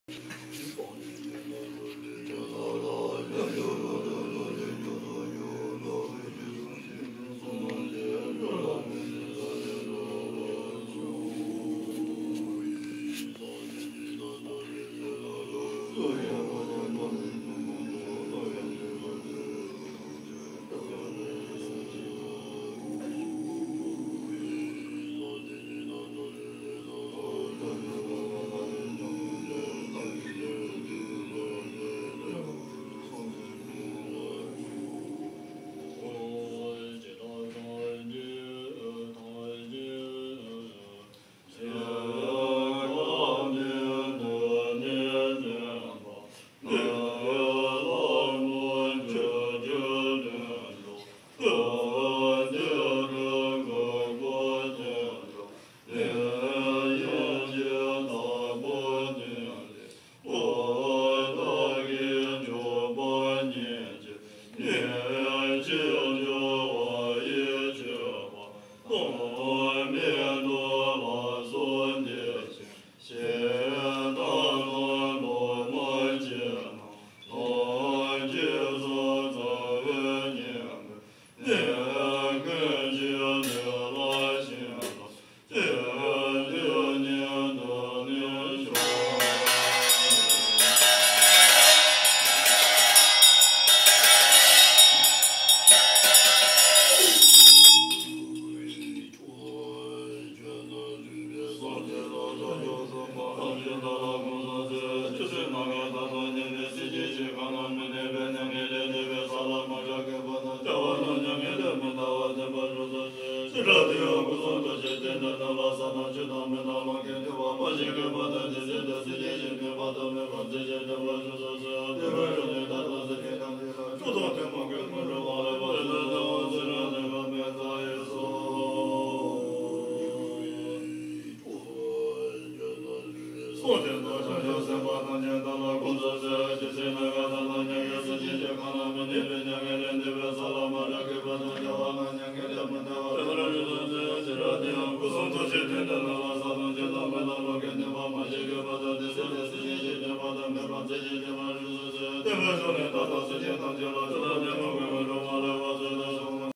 Tibetan Monks Mandala Closing Ceremony
Ackerman Middle School in Canby, Oregon had the amazing honor of welcoming 5 Tibetan monks to their school.  They spent three days creating a compassion mandala (sand painting).  This video is part of their closing ceremony.  It was an amazing experience for these middle schoolers and a great blessing for all.